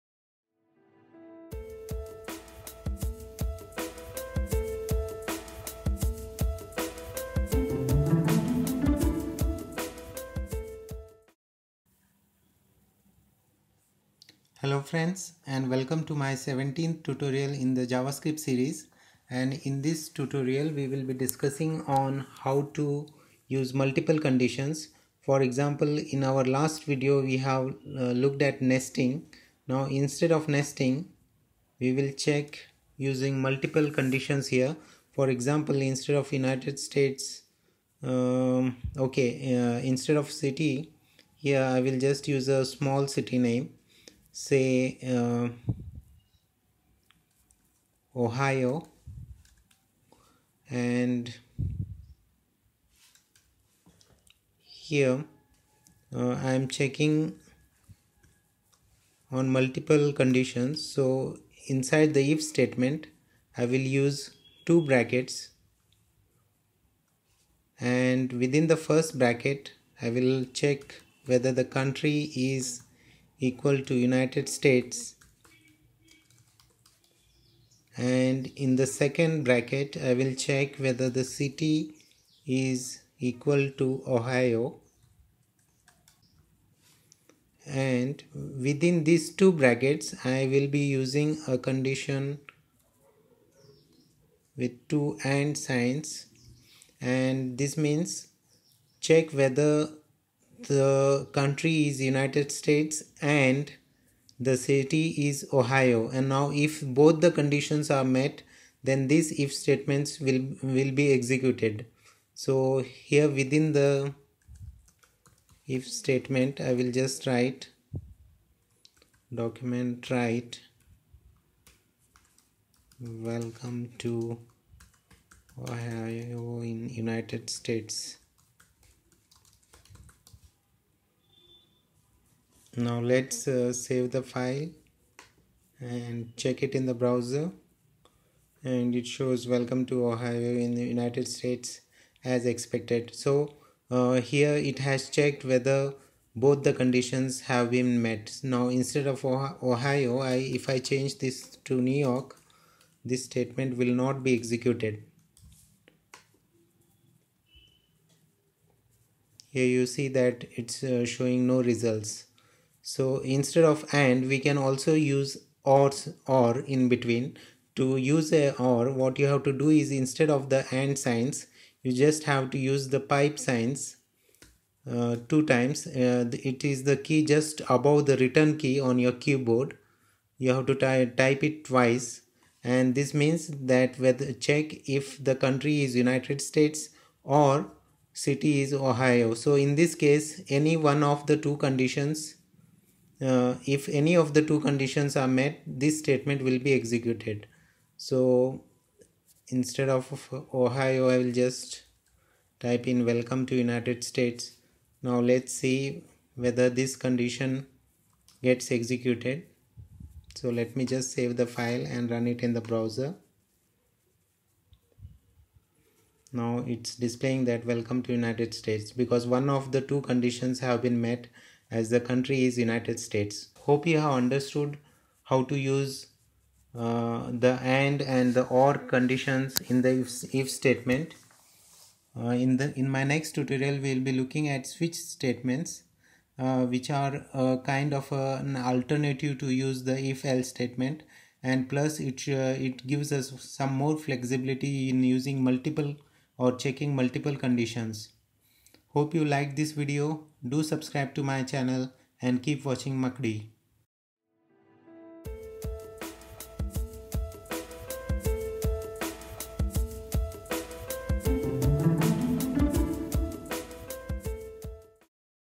JavaScript Tutorials for Beginners - Part 17 : Using Multiple Conditions in IF Statement
JavaScript Tutorials for Beginners discussing using multiple conditions in a IF ELSE statement.